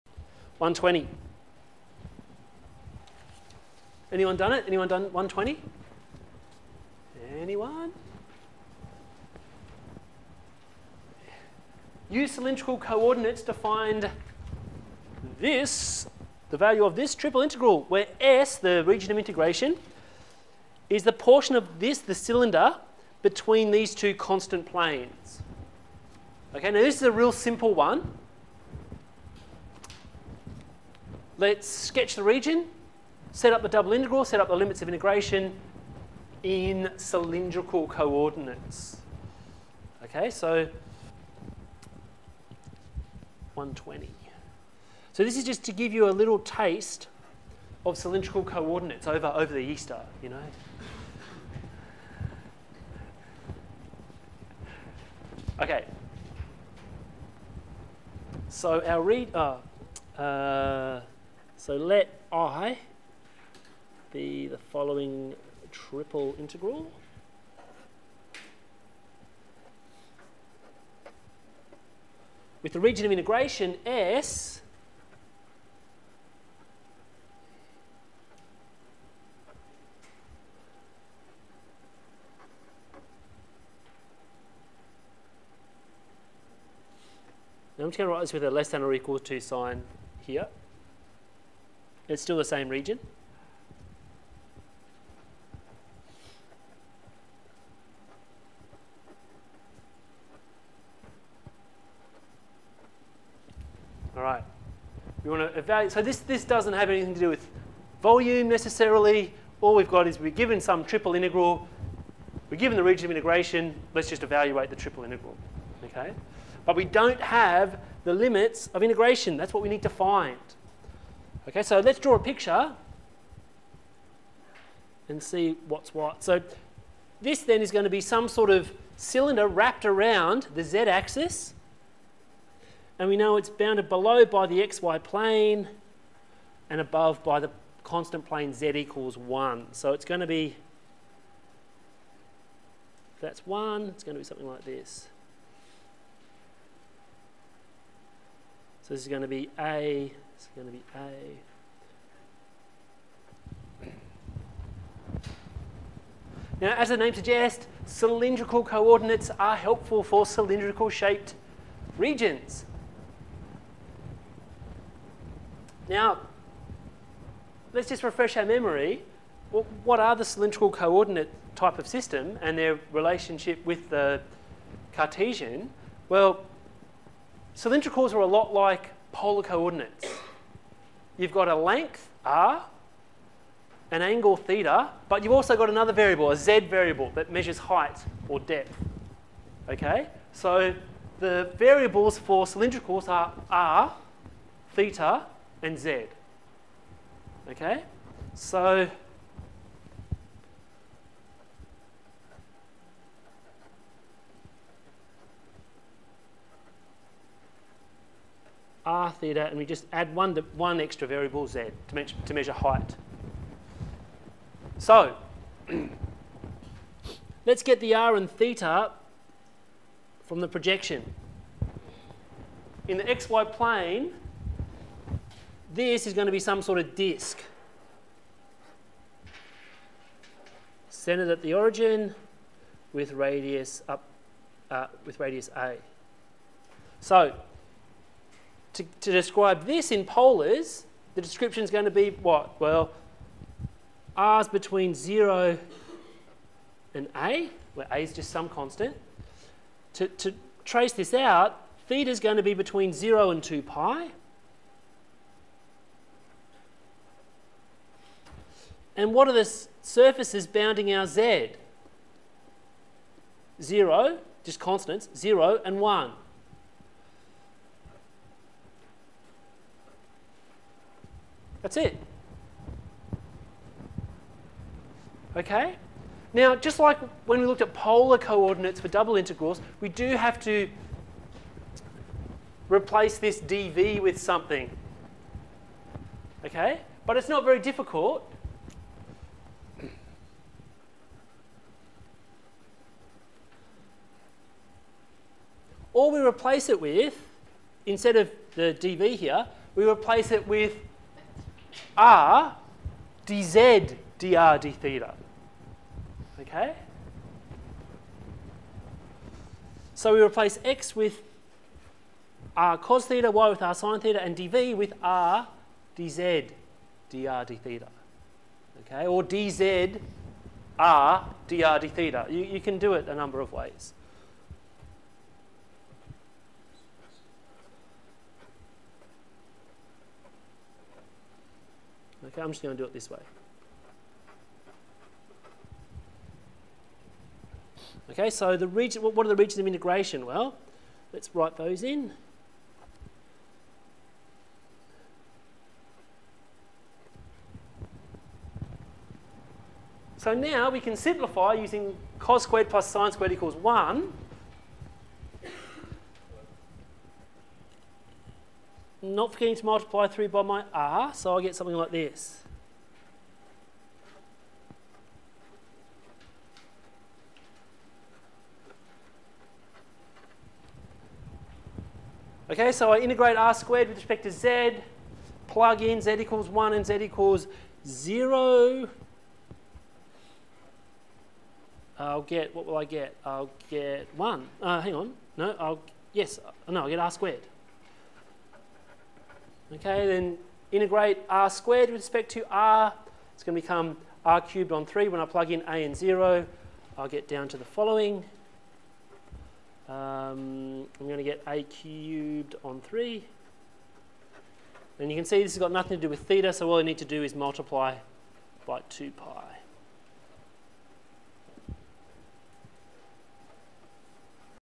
Triple integral in cylindrical co ordinates
Basic example of a triple integral integral in cylindrical co ordinates.  I show how to set it up and how to evaluate it.